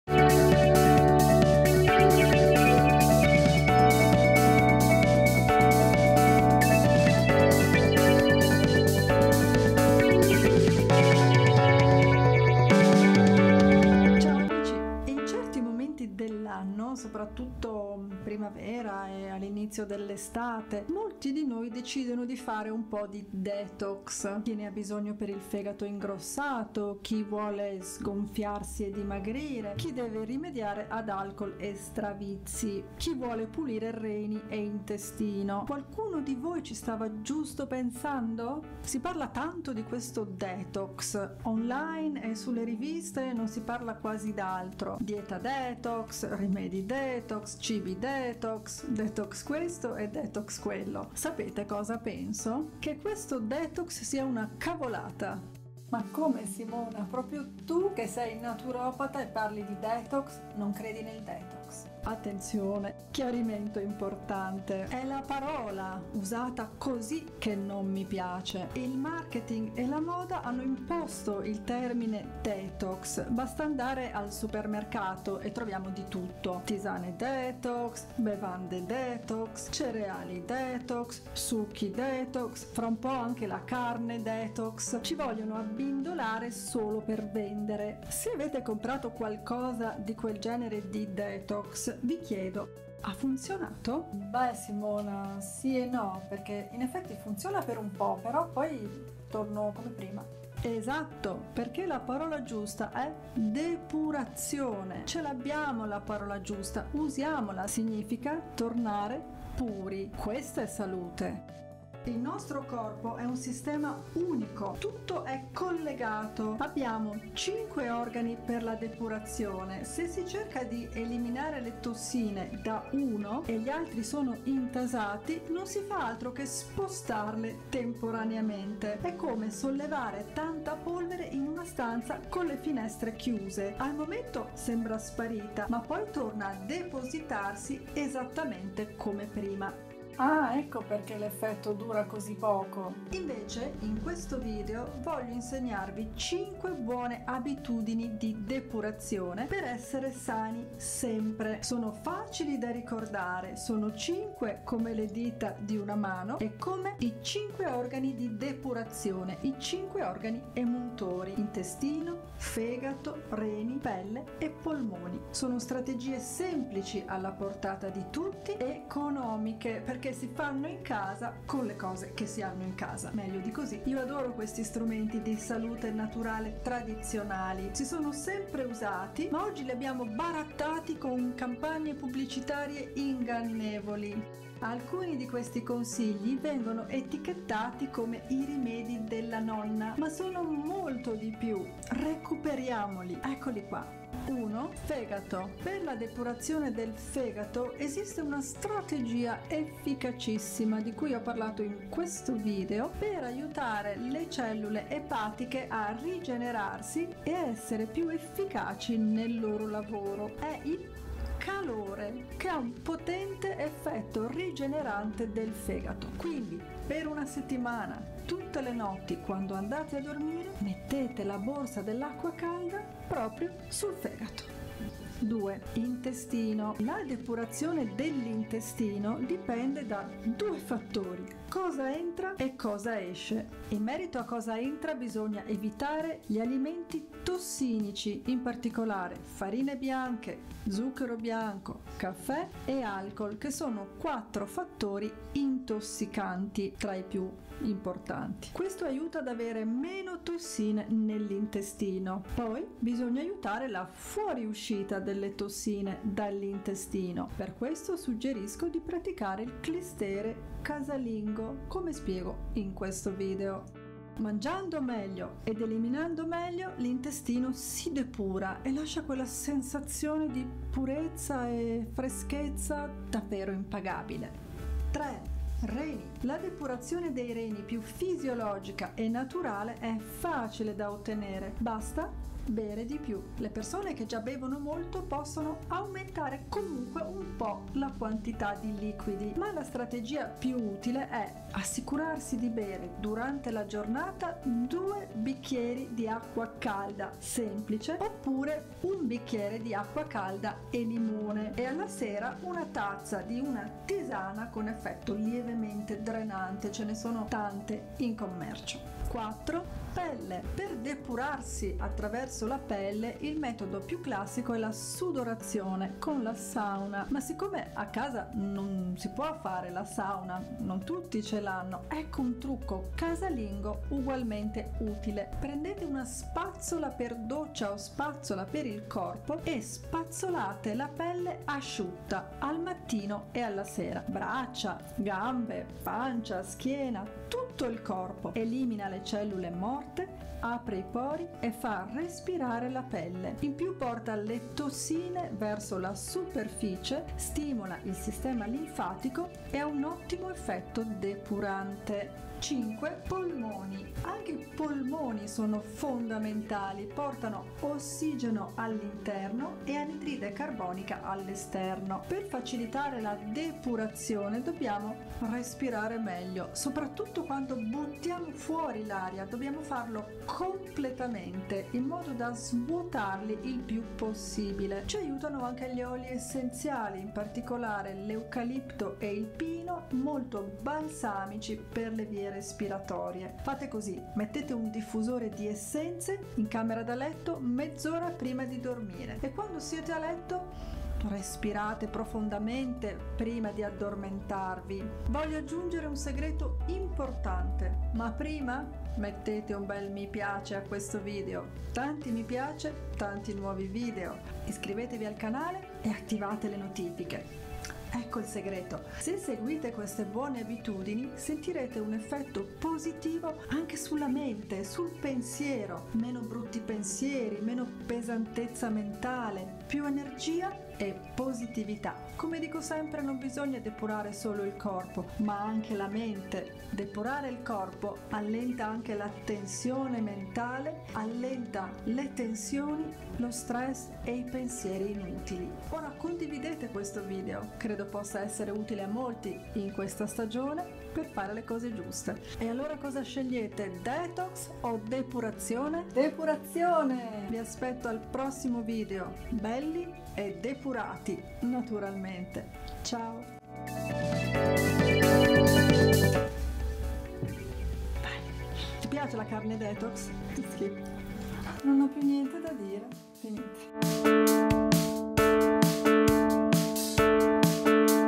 come DEPURARSI: DETOX o DEPURAZIONE? programma SETTIMANA DETOX con 5 TRUCCHI per DIMAGRIRE,FEGATO...
Fai un detox totale in 7 giorni, è semplice e veloce!

Questa è la soluzione più pratica, gustosa e adatta a tutti per depurare tutto il corpo in modo naturale ❇️

DETOX o DEPURAZIONE? come DEPURARSI per DIMAGRIRE, FEGATO, SGONFIARSI, lberare INTESTINO, PELLE, RENI

Detox VS Depurazione: voi da che parte state?

In questo video vi svelo la verità sulla detox e sulla depurazione. Innanzitutto, qual'è il termine corretto? E come funziona la depurazione, nell'organismo?

Poi vi svelo la mia strategia in 5 semplici passi per una depurazione totale, che possono diventare 5 ottime abitudini detox da seguire sempre..

Simona Vignali